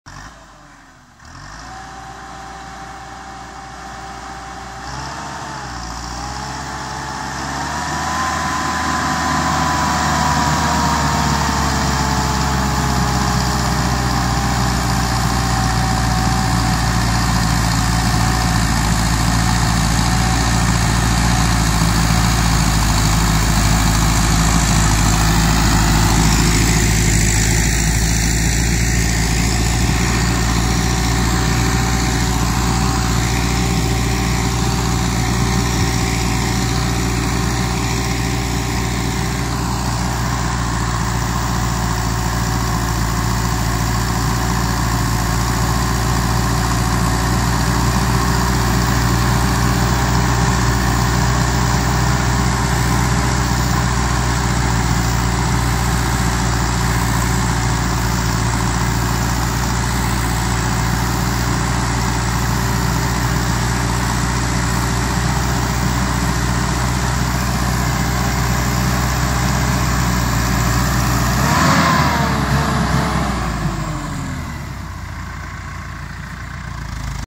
CASE-Karla Mücadele
Case jx65c traktörün 1 metre karda ilerleme performansı. Görüşlerinizi yorum kısmında görmek isterim.

İyi seyirler :)